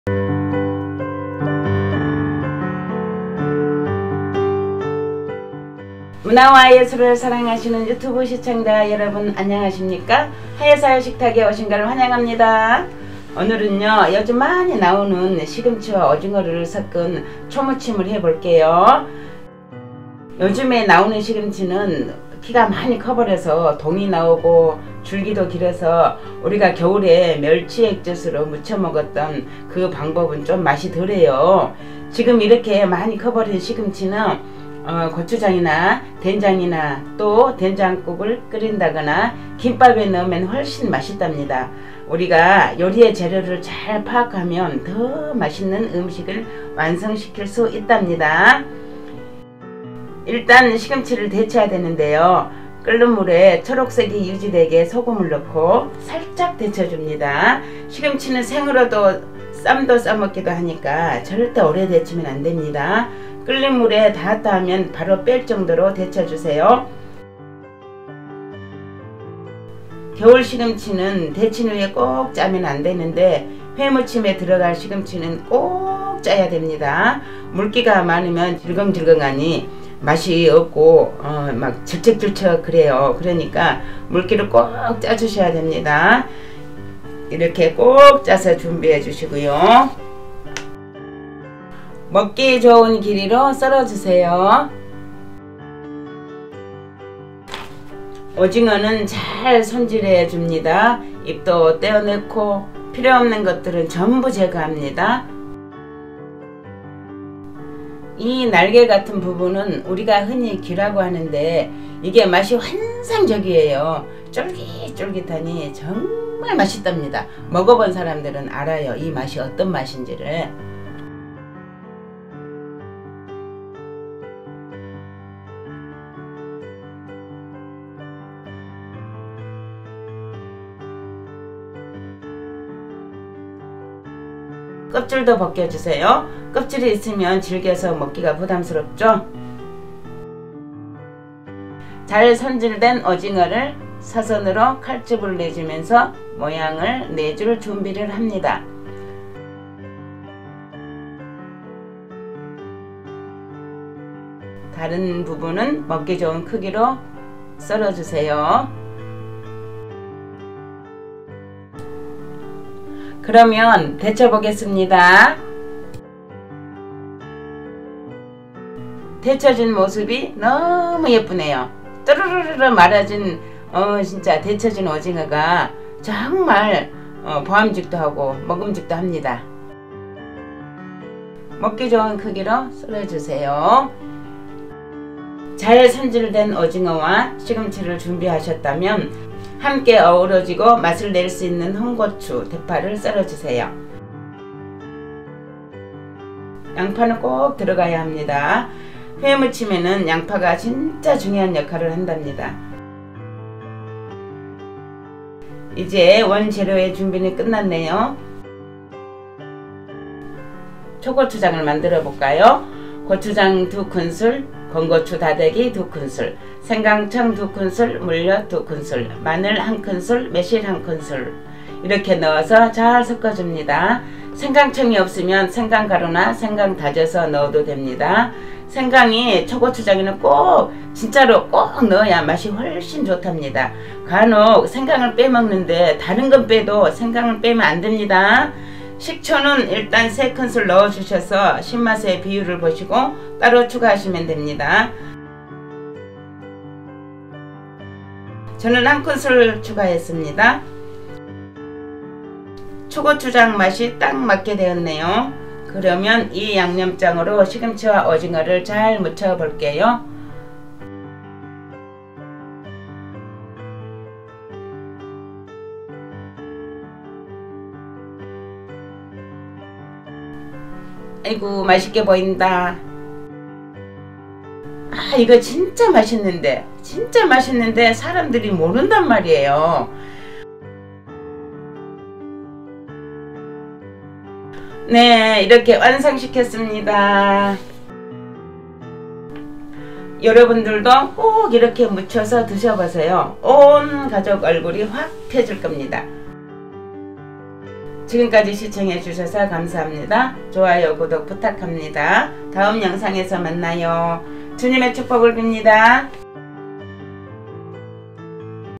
시금치와 오징어 초무침 : 먹어본 사람들은 모두 기절하는 맛!!! 오늘 이 회무침을 만들어 보세요. 가족들이 탄성을 지를 것입니다.
이른 봄에 나오는 동이 오른 시금치로 오징어와 함께 초 무침을 해 봤습니다.
이 초 무침을 모르는 사람이 많아 오늘 이렇게 해봅니다. 여러분도 맛있게 해서 드셔 보세요. 새로운 요리에 눈이 확 뜨일 것 입니다.

 - 재료-
1. 시금치     :  300g
2. 오징어     :  중 1마리
3. 양파        :  소 0.5 개
4. 홍 고추    :  2개
5. 대파        :  0.5 대

 - 양념 재료 -
1. 건 고추  다대기     : 2 Ts
2. 고추장                  : 2 Ts
3. 물엿                      : 2 Ts
4. 식초                      :  4 Ts
5. 매실액                  : 1 Ts
6. 생강청                  : 2 Ts
7. 마늘                      : 1 Ts
8. 깨소금                  : 1 Ts